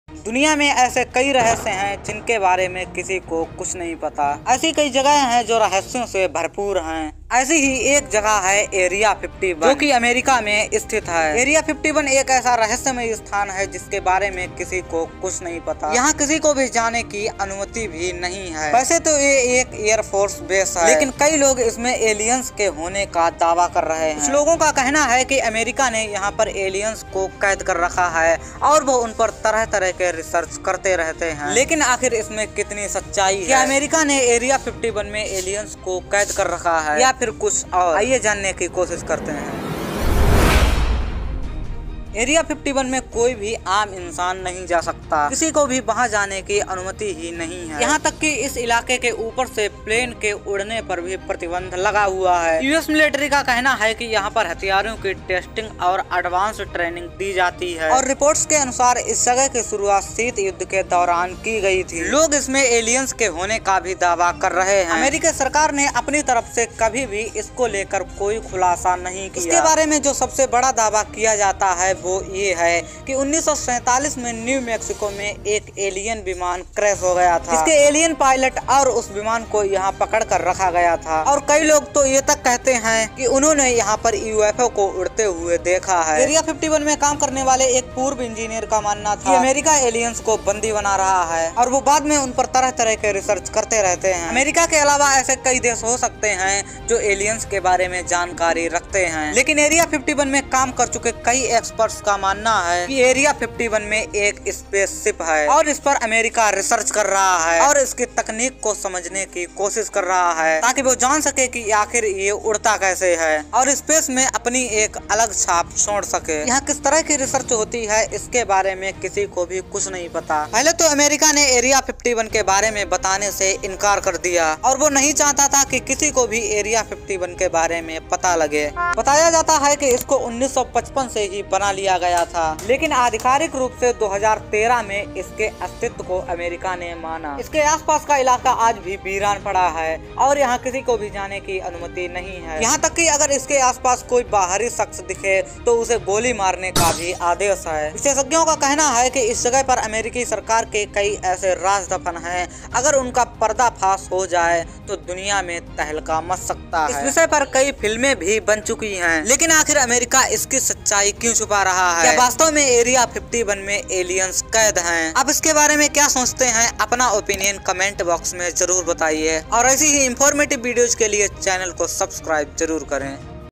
खुल गया Area51 का राज? क्या यहाँ कैद हैं एलियन?👽 Logical Boy
kya area 51 me alien hai?
area51 ka raj
area 51 kya hai